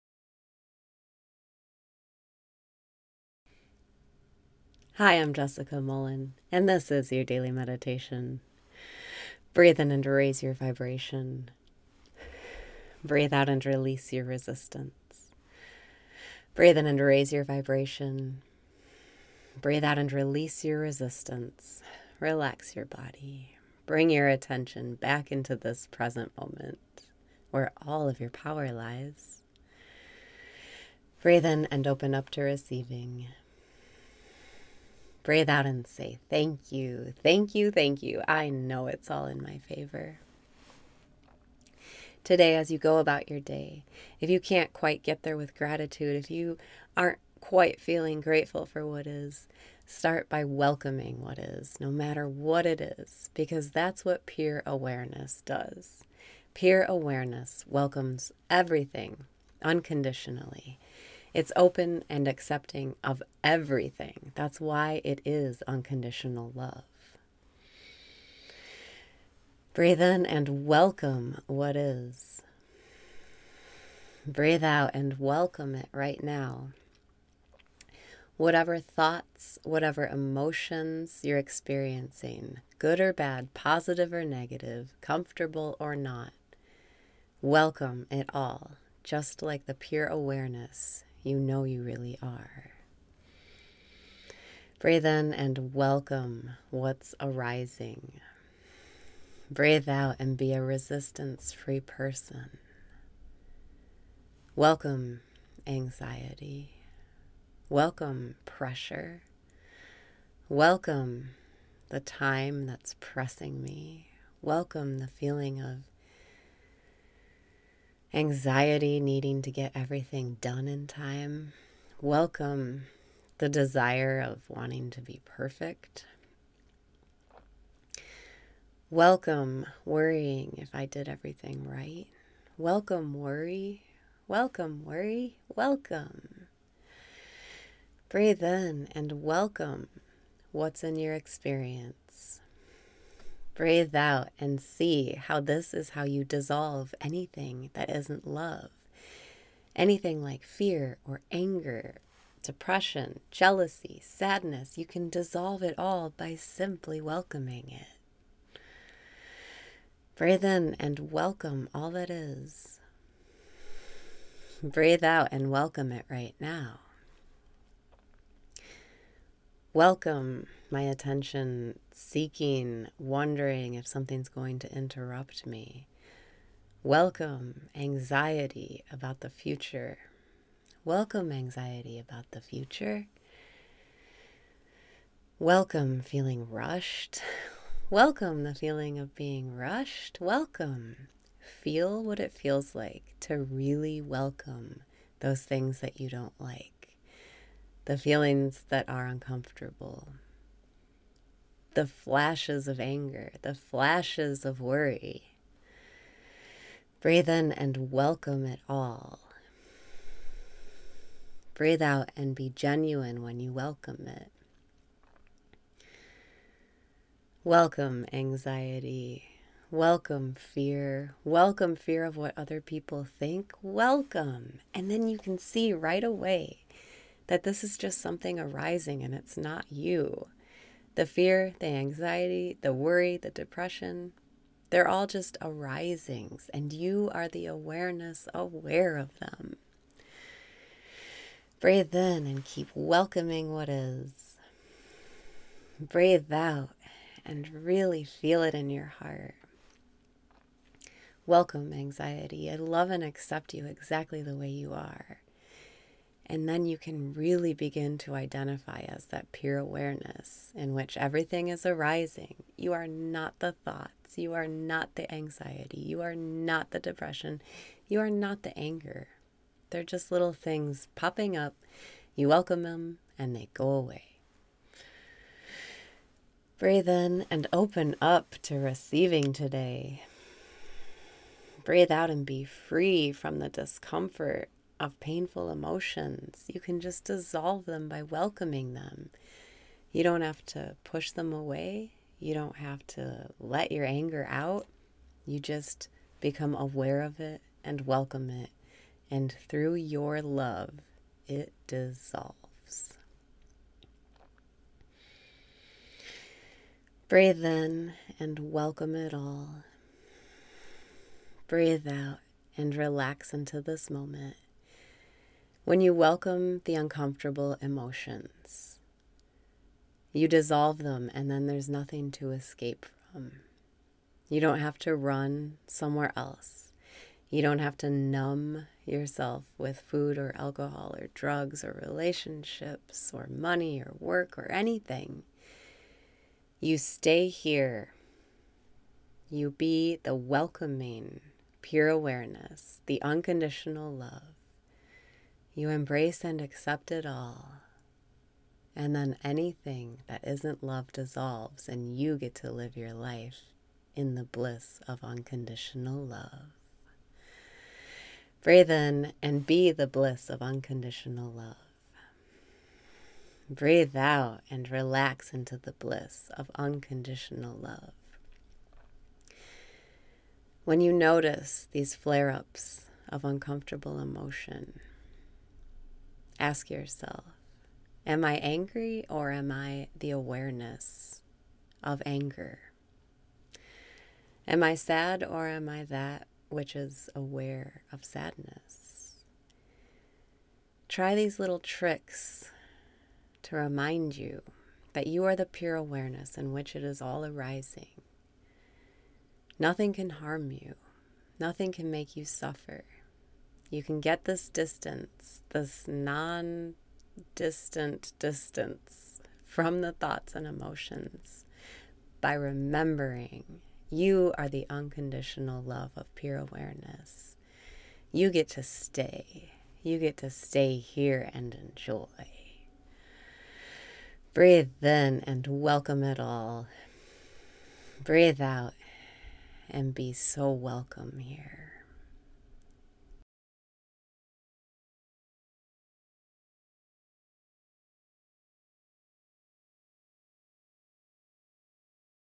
Daily Meditation - Welcome Worry
Relax your body. Bring your attention back into this present moment, where all of your power lies.

Breathe out and say, Thank you. Thank you. Thank you. I know it's all in my favor.

Today, as you go about your day, if you can't quite get there with gratitude, if you aren't quite feeling grateful for what it is, start by welcoming what is, no matter what it is, because that's what pure awareness does.

Pure awareness welcomes everything unconditionally. It's open and accepting of everything. That's why it is unconditional love.

Breathe in and welcome what is.
Breathe out and welcome it right now.

Whatever thoughts, whatever emotions you're experiencing, good or bad, positive or negative, comfortable or not—welcome it all, just like the pure awareness you know you really are.

Breathe in and welcome what's arising.
Breathe out and be a resistance-free person.

Welcome anxiety. Welcome pressure. Welcome the time that's pressing me. Welcome the feeling of anxiety, needing to get everything done in time. Welcome the desire of wanting to be perfect. Welcome worrying if I did everything right. Welcome worry. Welcome worry. Welcome.

Breathe in and welcome what's in your experience.
Breathe out and see how this is how you dissolve anything that isn't love.

Anything like fear or anger, depression, jealousy, sadness—you can dissolve it all by simply welcoming it.

Breathe in and welcome all that is.

Welcome my attention seeking, wondering if something's going to interrupt me. Welcome anxiety about the future. Welcome anxiety about the future. Welcome feeling rushed. Welcome the feeling of being rushed. Welcome. Feel what it feels like to really welcome those things that you don't like, the feelings that are uncomfortable, the flashes of anger, the flashes of worry.

Breathe in and welcome it all.
Breathe out and be genuine when you welcome it.

Welcome anxiety. Welcome fear. Welcome fear of what other people think. Welcome and then you can see right away that this is just something arising and it's not you. The fear, the anxiety, the worry, the depression—they're all just arisings and you are the awareness aware of them.

Breathe in and keep welcoming what is.
Breathe out and really feel it in your heart.

Welcome anxiety. I love and accept you exactly the way you are. And then you can really begin to identify as that pure awareness in which everything is arising.

You are not the thoughts. You are not the anxiety. You are not the depression. You are not the anger. They're just little things popping up. You welcome them and they go away.

Breathe out and be free from the discomfort of painful emotions.

You can just dissolve them by welcoming them. You don't have to push them away. You don't have to let your anger out. You just become aware of it and welcome it and through your love, it dissolves.

Breathe out and relax into this moment.

When you welcome the uncomfortable emotions, you dissolve them and then there's nothing to escape from. You don't have to run somewhere else. You don't have to numb yourself with food or alcohol or drugs or relationships or money or work or anything.

You stay here. You be the welcoming, pure awareness, the unconditional love. You embrace and accept it all. And then anything that isn't love dissolves and you get to live your life in the bliss of unconditional love.

Breathe in and be the bliss of unconditional love.
Breathe out and relax into the bliss of unconditional love.

When you notice these flare ups of uncomfortable emotion, ask yourself, "Am I angry or am I the awareness of anger? Am I sad or am I that which is aware of sadness?" Try these little tricks to remind yourself that you are the pure awareness in which it is all arising.

Nothing can harm you. Nothing can make you suffer. You can get this distance, this non-distant distance from the thoughts and emotions by remembering you are the unconditional love of pure awareness.

You get to stay. You get to stay here and enjoy.

Breathe then and welcome it all.
Breathe out and be so welcome here.